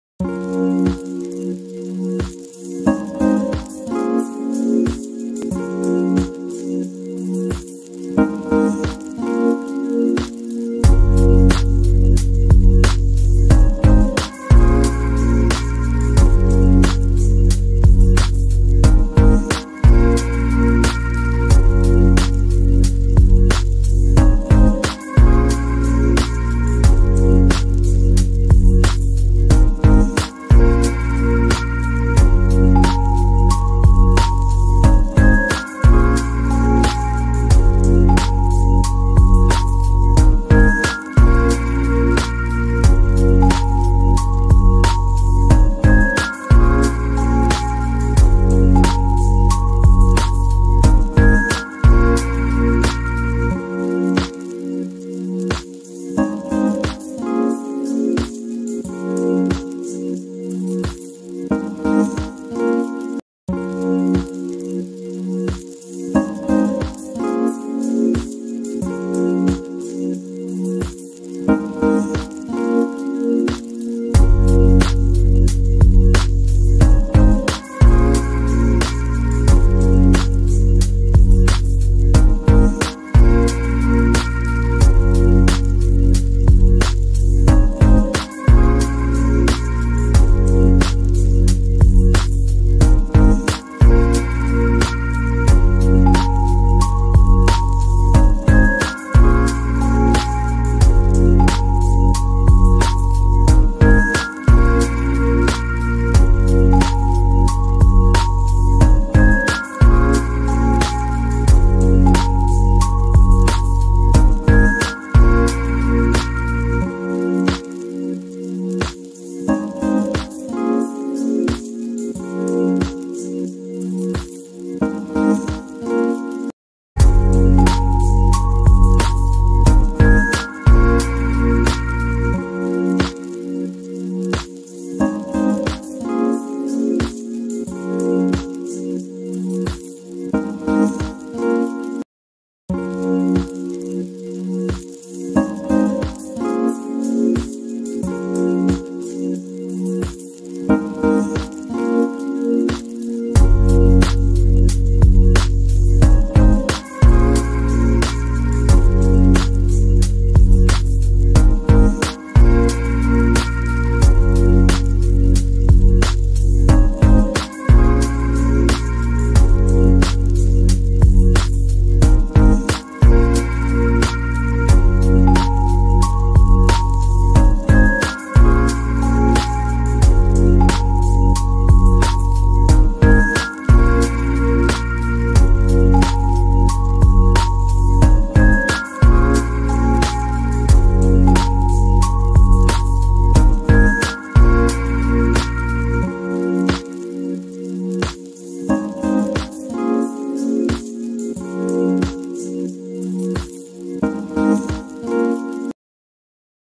1कटोरी दही और सूजी से बनाये हेल्दी नाश्ता- Vegetable Idli Recipe-have 39% calories#cookingbismillah
1 कटोरी दही और सूजी से बनाये हेल्दी नाश्ता- Vegetable Idli Recipe | Instant  idli |have 39% calories